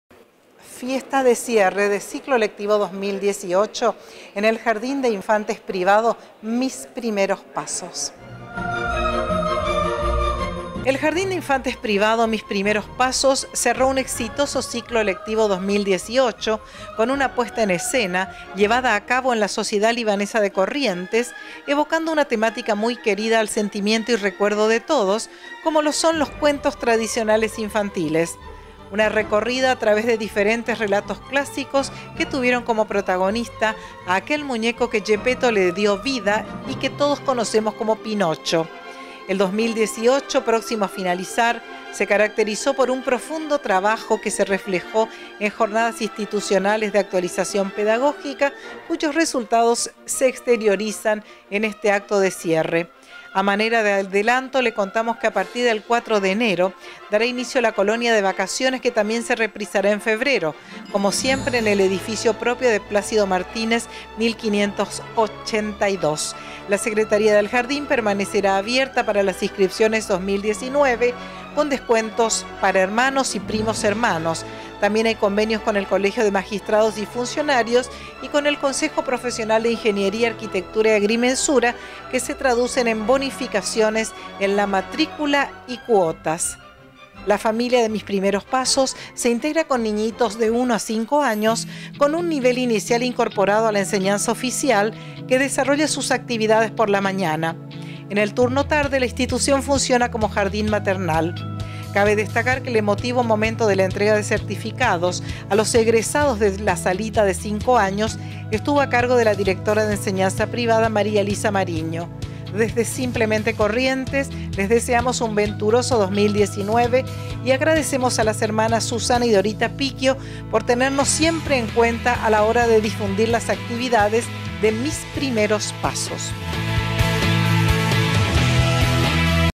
Jardín Mis Primeros Pasos
Fiesta de Fin de Año realizada en la Sociedad Libanesa de Corrientes